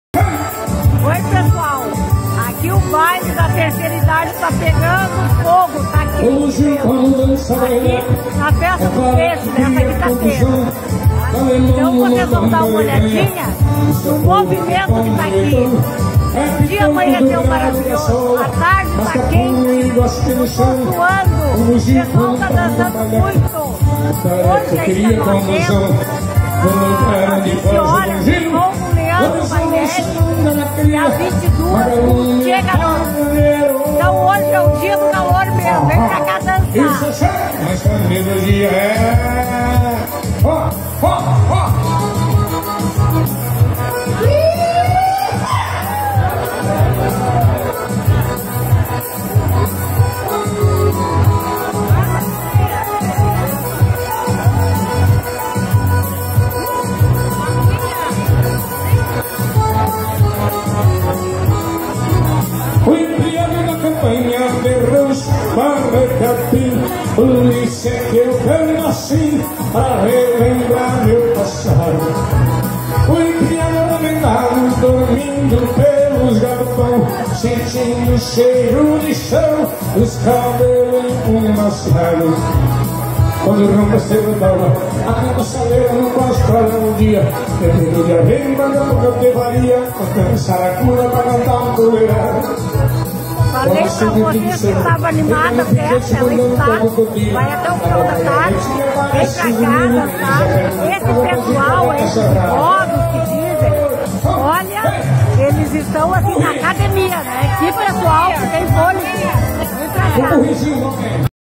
Quinta-feira quente na 32ª Festa Nacional do Peixe.
Quinta-feira quente na 32ª Festa Nacional do Peixe, com Baile na 3ª idade animado por Paulista Show.